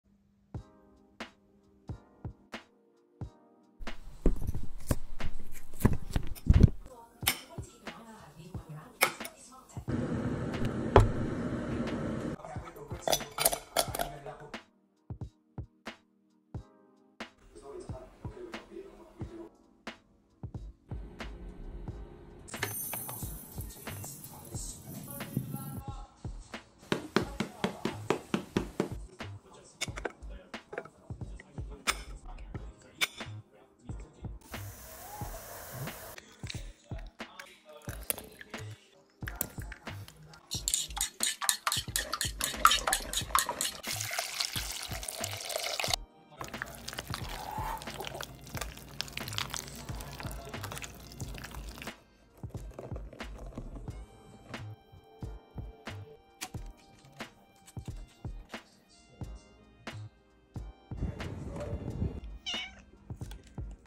Brownies 🍫
Join us in the kitchen as our talented feline chef whips up a batch of the most delicious chocolate brownies you've ever seen! 🐱👩‍🍳 From mixing the ingredients to the final, mouthwatering result, this adorable cat knows its way around the kitchen. Get ready for a purr-fectly sweet treat that will leave you craving for more! 🎥🍽️